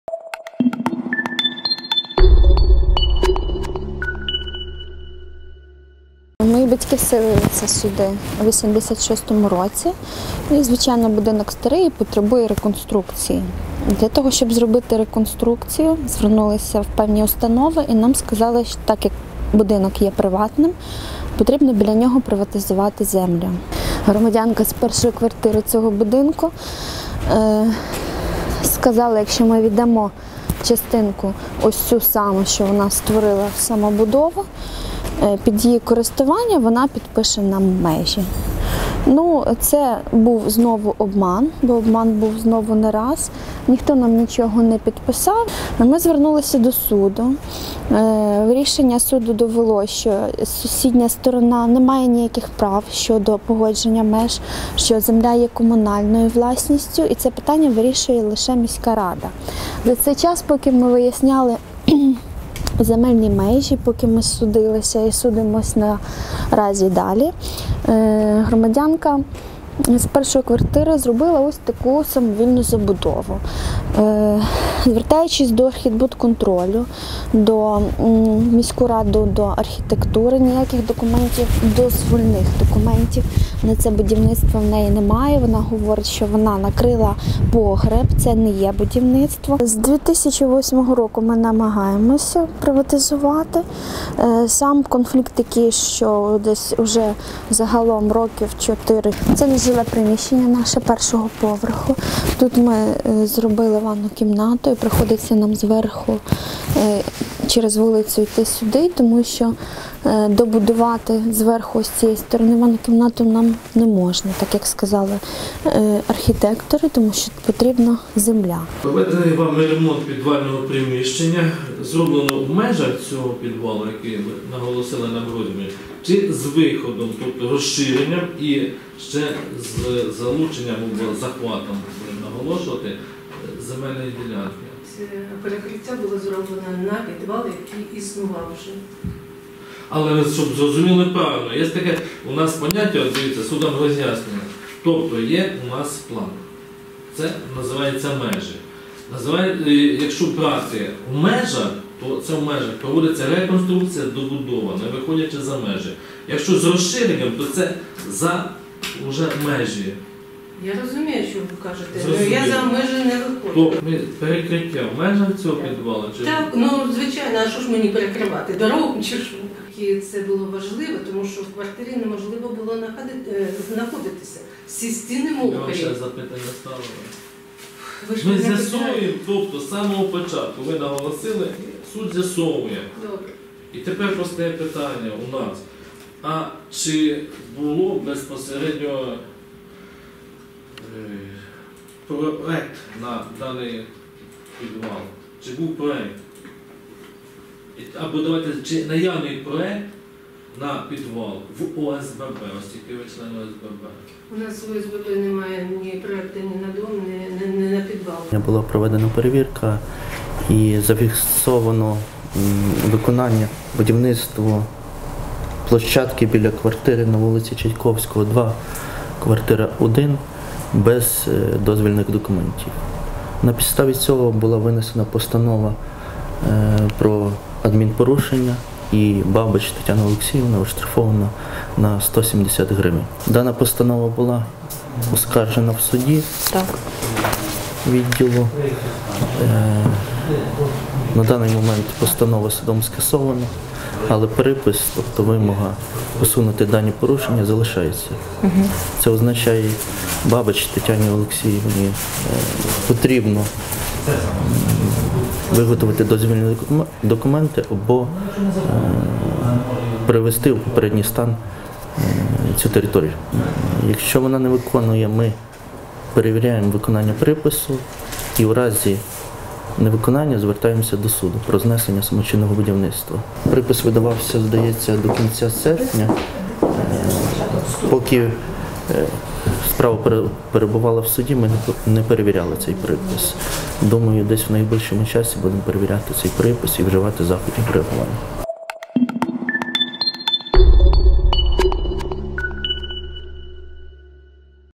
Лучанка чотири роки «воює» за рідне подвір’я
Лучанка Наталія Манн живе у будинку, який дістався їй від батьків. Її сім’я мешкає на другому поверсі будівлі, бо перший поверх дому не придатний до життя. Там необхідно робити капітальний ремонт та реконструкцію.